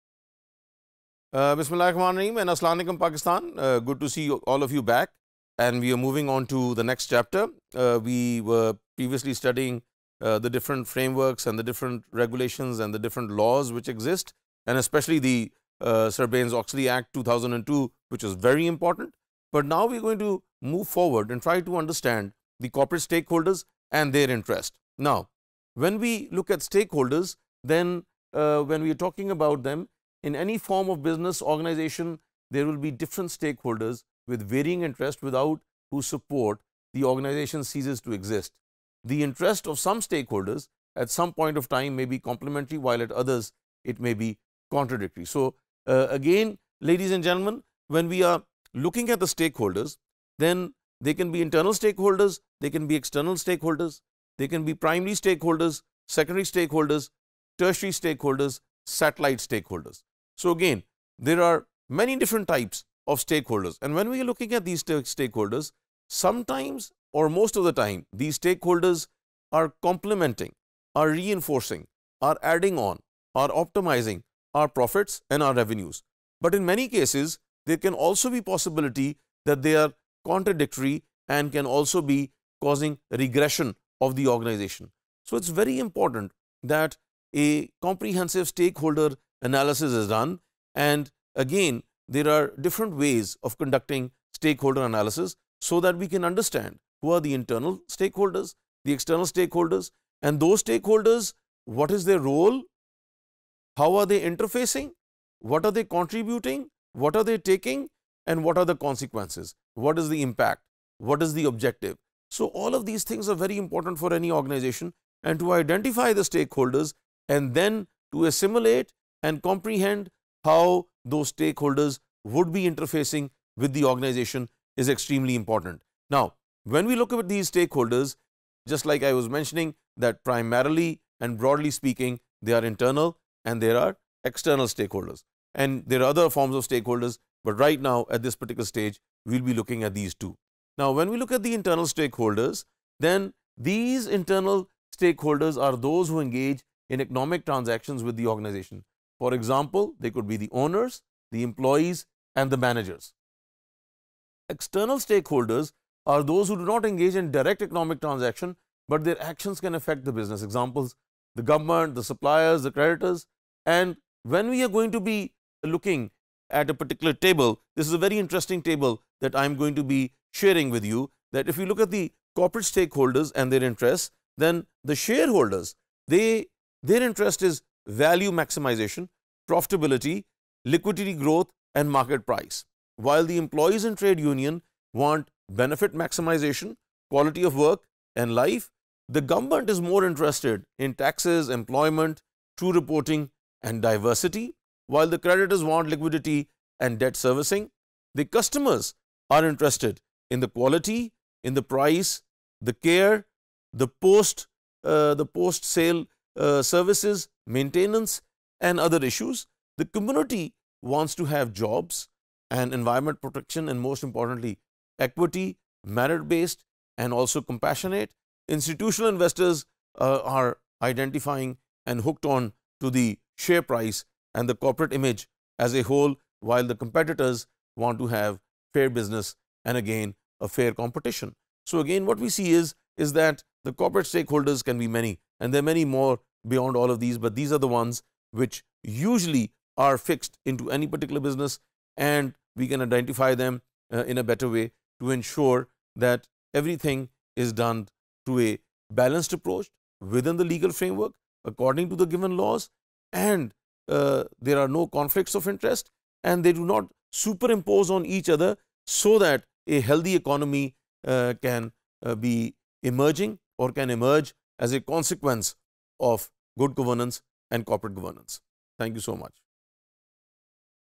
Corporate Stake Holders and Their Interest | Corporate Governance | MGT717_Topic082
Topic082: Corporate Stake Holders and Their Interest,
By Syed Hussain Haider